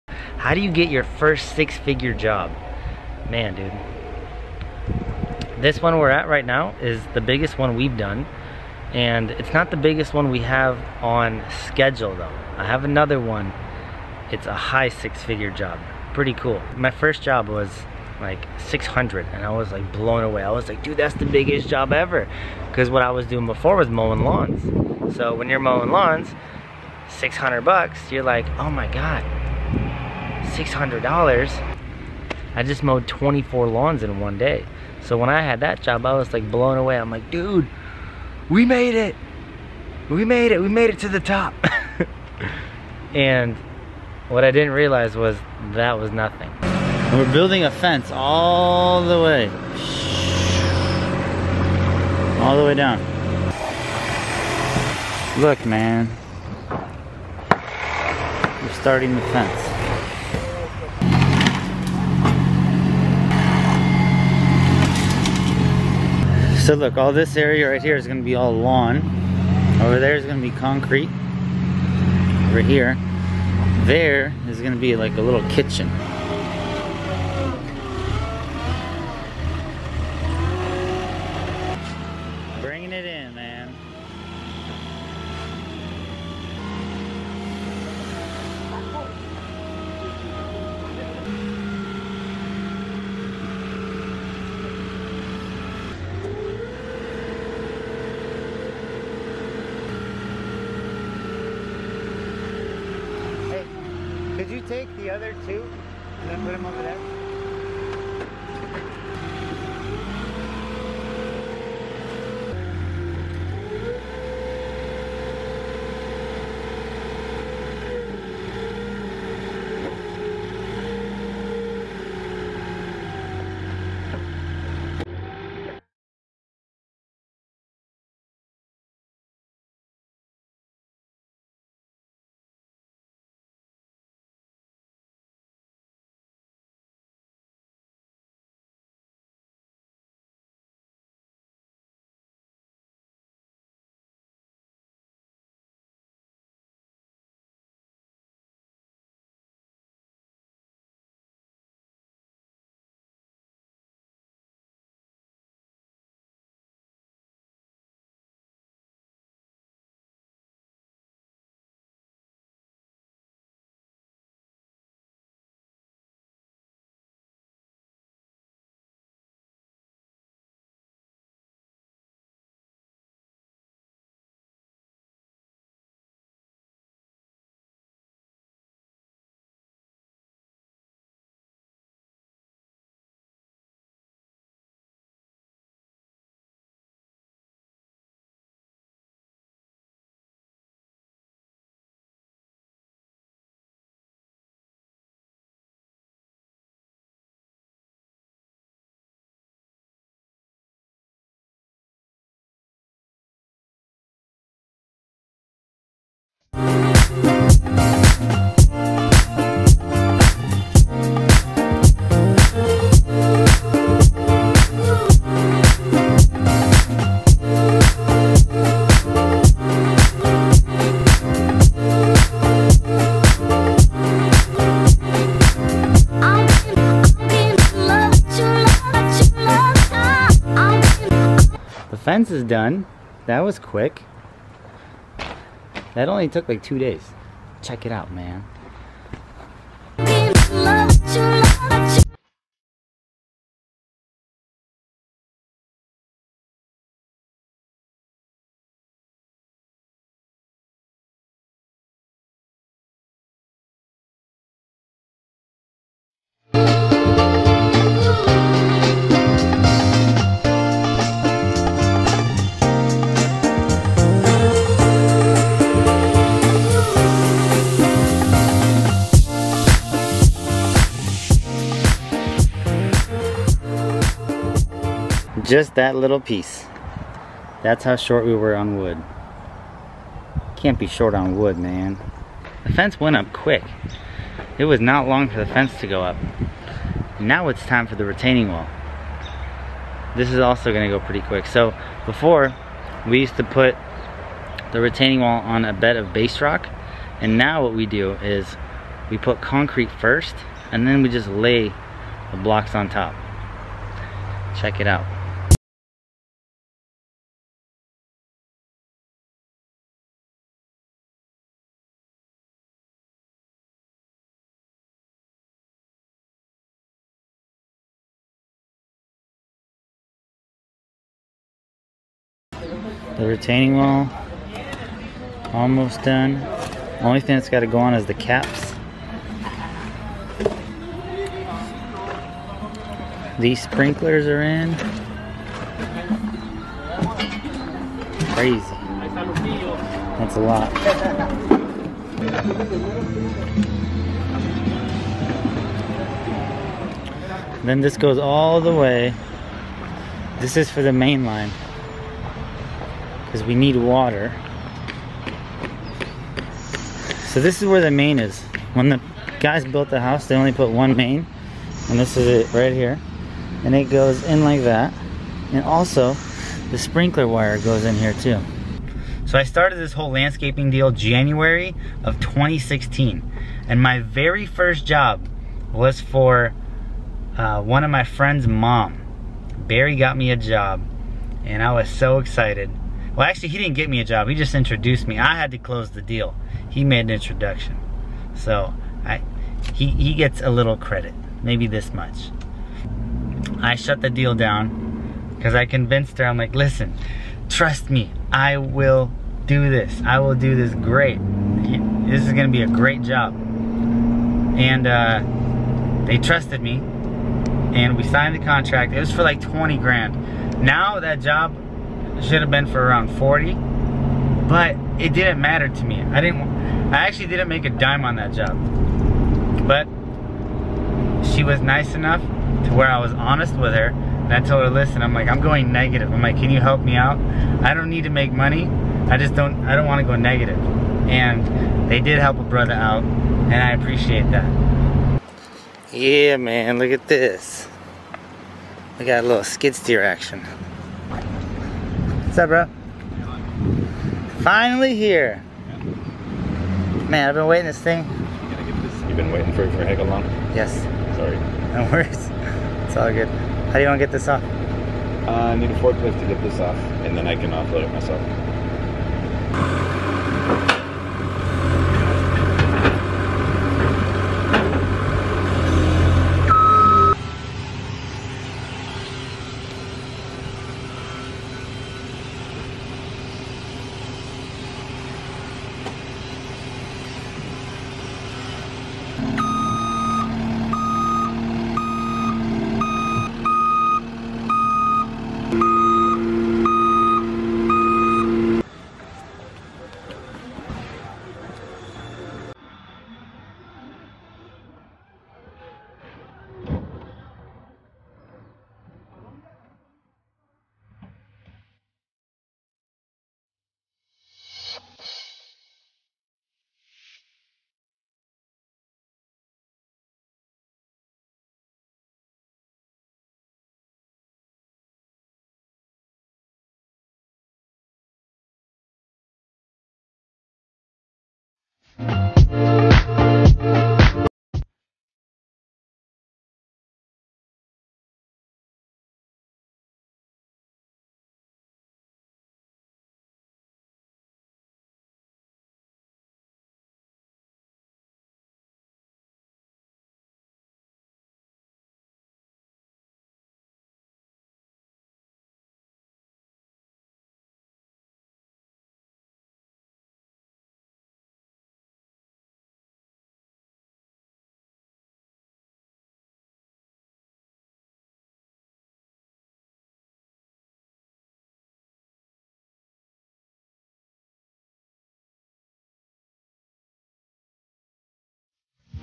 How To Get Big Landscaping Jobs $100,000+
I got my first 6 figure job. This one is a huge risk.